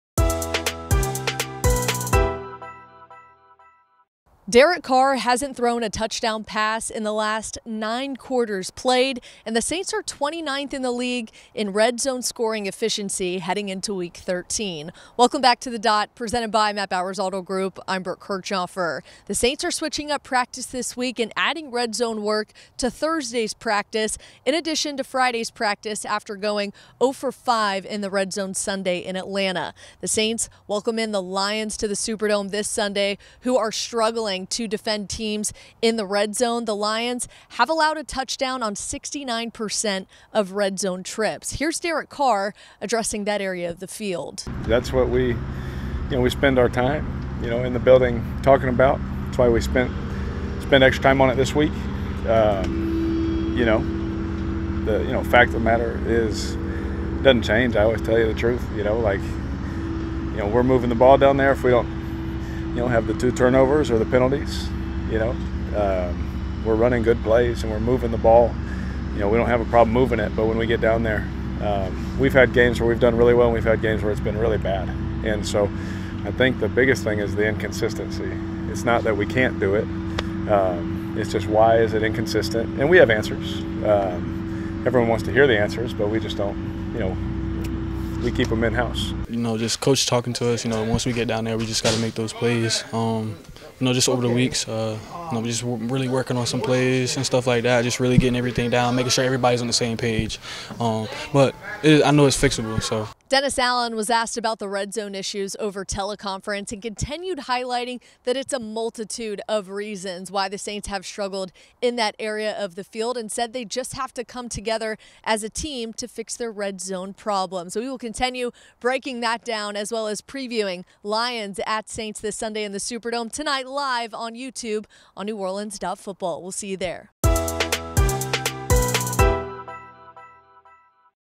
How will the Saints offense improve in the red zone?
Brooke recaps the Saints continued red zone struggles and breaks down the steps being taken to improve going into Sunday's game against Detroit